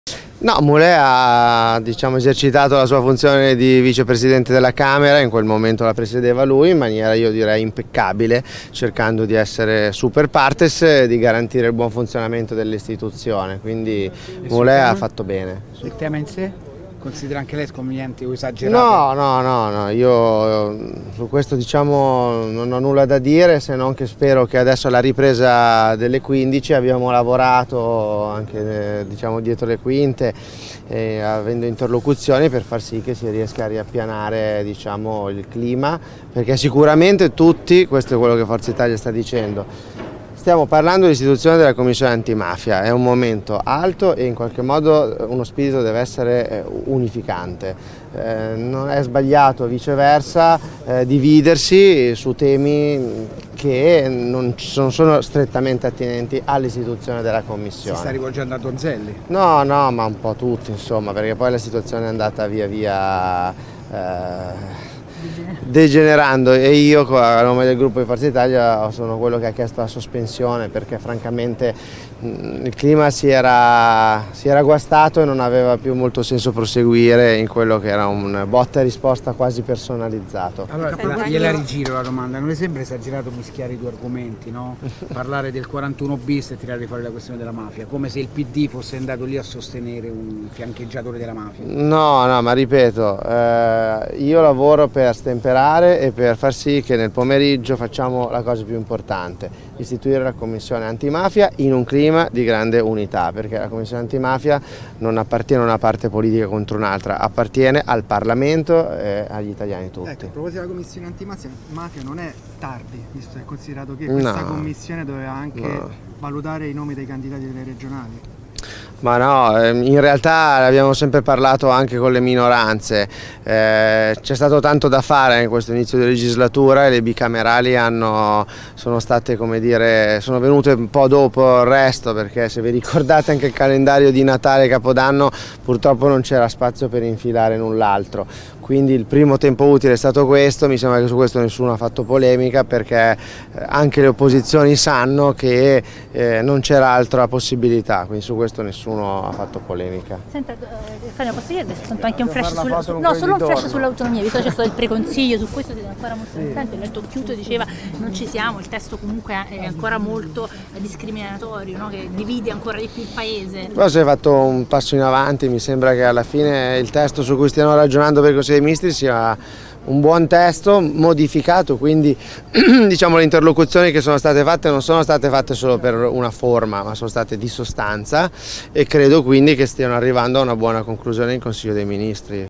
Antimafia, Cattaneo: "Non è tardi, abbiamo avuto tanto da fare in questo inizio legislatura"
Sulle autonomie: "fatto in passo in avanti. Testo buono"

POLITICA (Roma). "Mulé ha esercitato il suo ruolo da vicepresidente della Camera ed ha cercato di essere superpartes. D'altronde, presiedeva l'aula lui e ha voluto garantire il buon funzionamento delle istituzioni. Io sul tema Cospito non ho nulla da dire. Spero che con la ripresa dell'aula si riappiani il clima. Tutti dobbiamo lavorare per la commissione antimafia ed è sbagliato dividerci ora". Lo ha detto il Capogruppo di Forza Italia alla Camera, Alessandro Cattaneo, intercettato fuori Montecitorio. "Non è tardi per la commissione. Abbiamo sempre parlato con le minoranze ma abbiamo avuto tanto da fare in questo inizio legislatura. Il primo tempo utile è stato questo, anche le opposizioni sanno che non c'era latra possibilità", ha continuato. Mentre sulle autonomie ha detto: "è stato fatto un passo in avanti. Il testo su cui stiamo ragionando è buono". (Davide Di Carlo/alanews)

No amore diciamo esercitato la sua funzione di vice presidente della Camera in quel momento la preside va lui in maniera Io direi impeccabile cercando di essere super partes di garantire il funzionamento dell'istituzione quindi voleva fatto bene No no no no io non ho nulla da dire che spero che adesso la ripresa delle 15 abbiamo lavorato anche diciamo dietro le quinte e avendo interlocuzioni Per far sì che si riesca a appianare diciamo il clima perché sicuramente tutti questo è quello che ha fatto Italia sta dicendo stiamo parlando l'istituzione della commissione antimafia è un momento alto e in qualche modo uno spillo deve essere unificante non è sbagliato viceversa dividerci su temi che non ci sono sono strettamente attinenti al istituzione della commissione  tutto insomma perché poi la situazione è andata via via degenerando e io non me le gruppo e farti Italia sono quello che ha chiesto a sospensione perché francamente prima si era si era guastato e non aveva più molto senso proseguire in quello che era una botta e risposta quasi personalizzato documenti No questo parlare del 41-bis e tirare fuori la questione della mafia come se il PD fosse andato lì a sostenere un campeggiatore della mamma no no Ma ripeto io lavoro per stemperare e per far sì che nel pomeriggio facciamo la cosa più importante istituire la commissione antimafia In un clima di grande unità perché la commissione antimafia non appartiene una parte politica contro un'altra appartiene al Parlamento e agli italiani tutti  realtà l'abbiamo sempre parlato anche con le minoranze c'è stato tanto da fare in questo inizio di legislatura e le bicamerali anno sono state Come dire sono venute un po' dopo festa perché se vi ricordate anche il calendario di Natale Capodanno purtroppo non c'era spazio per infilare in un l'altro quindi il primo tempo utile stato questo mi sono già su questo nessuno ha fatto polemica perché anche le opposizioni sanno che è non c'era altra possibilità qui su questo nessuno ha fatto polemica non ci siamo stesso Comunque hai ancora molto è discriminatorio cosa hai fatto un passo in avanti Mi sembra che alla fine è il testo su cui stiamo giocando per conto dei mistici a un buon testo modificato quindi diciamo l'interlocuzione che sono state fatte non sono state fatte solo per una forma ma sono state di sostanza e credo Quindi che  arrivando una buona conclusione in consiglio dei